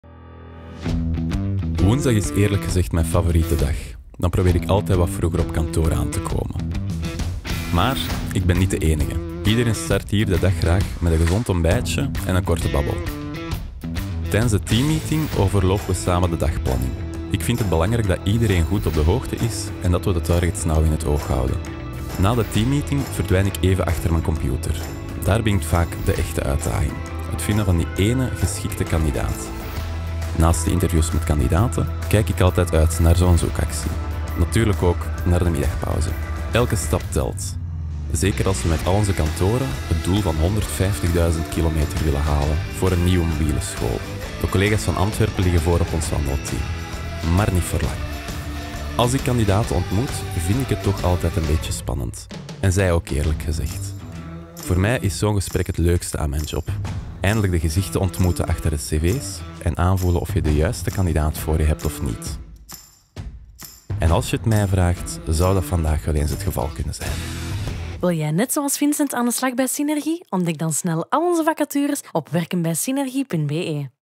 Bij Synergie kan je elke dag jezelf zijn | Vincent
Na een zoektocht eindelijk het gezicht achter het cv ontmoeten en de perfecte kandidaat vinden, dat maakt de werkdag van Vincent een groot succes.

Wil jij net zoals Vincent aan de slag bij Synergie? Ontdek dan snel al onze vacatures op